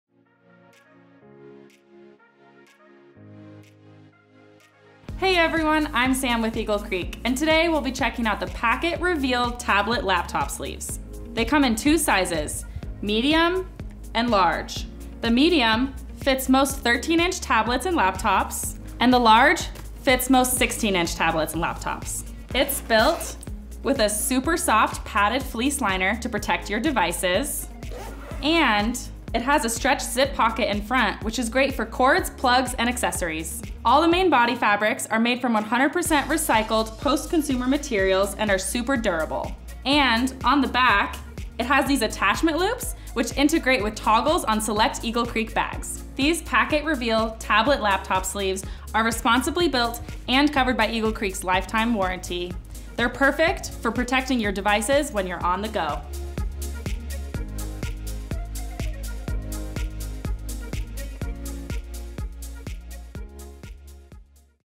Pack-It™ Reveal Tablet/Laptop Sleeves I Eagle Creek
These padded, fleece-lined sleeves are great for a tablet or laptop. This sleeve offers sleek protection of your electronics when storing in any travel bag. And get this: there’s a bonus zippered pocket that keeps all your mini accessories close-at-hand.